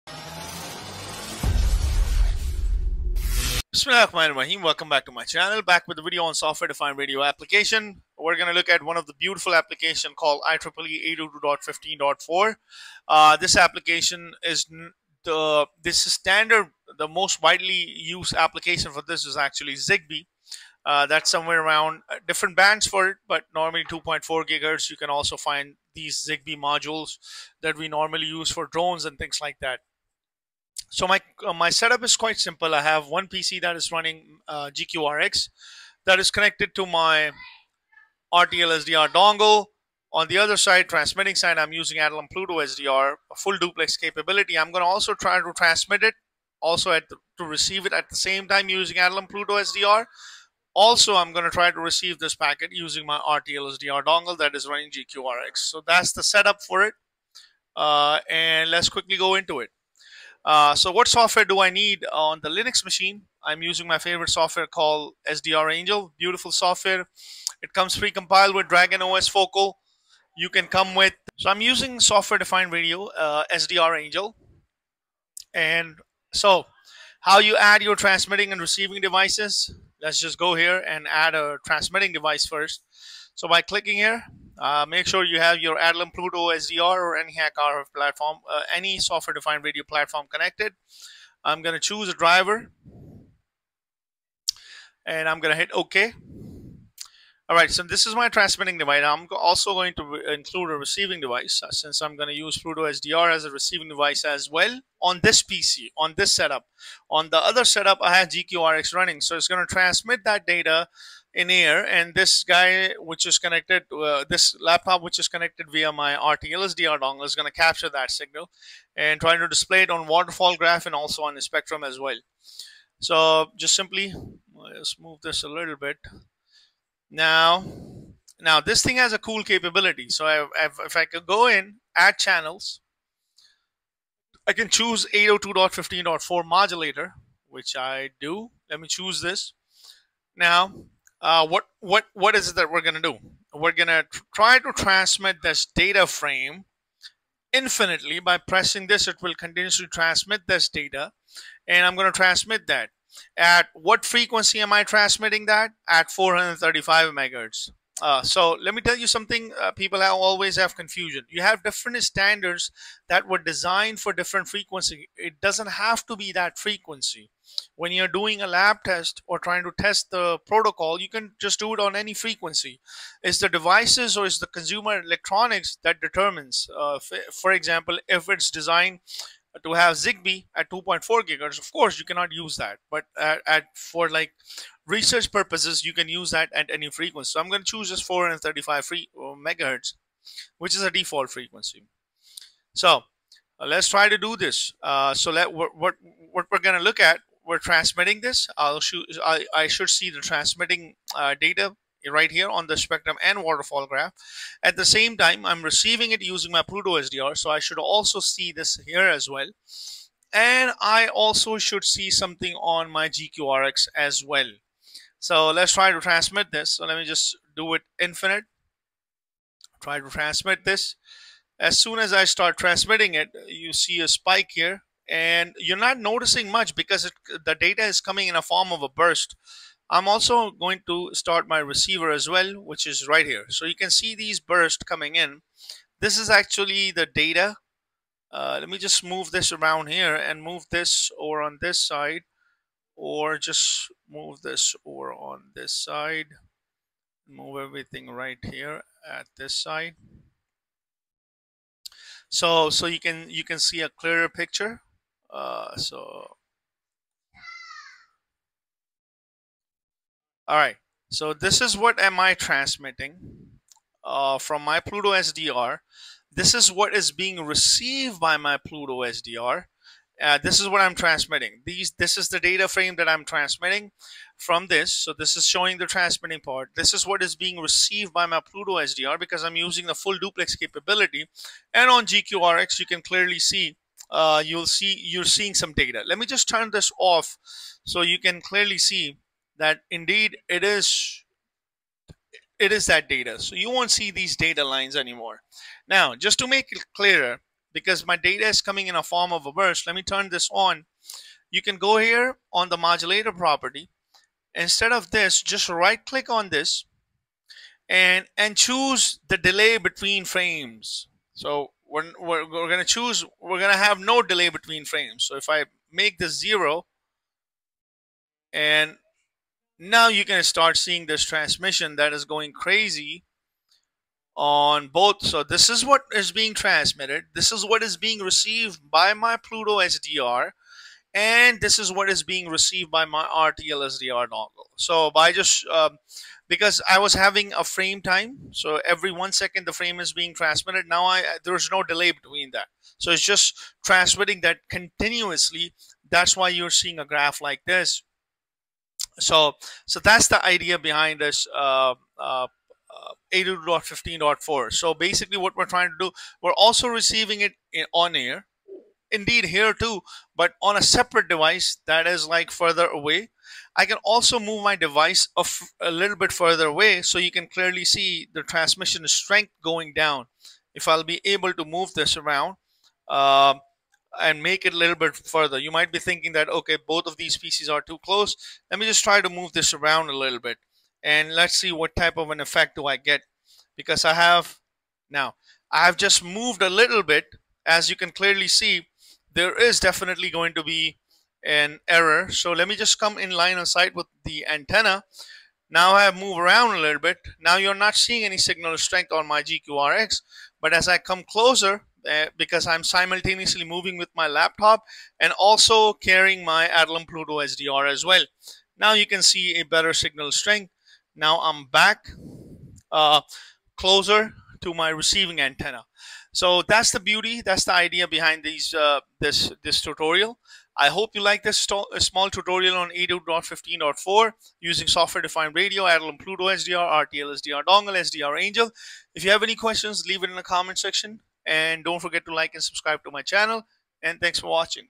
ZigBee IEEE 802.15.4 Transmission and Reception using PlutoSDR and RTL-SDR | SDRangel & GQRX
I transmitted 802.15.4 frame using SDRangel & PlutoSDR in full duplex mode. I received the same signal using rtl-sdr which was running on a different laptop.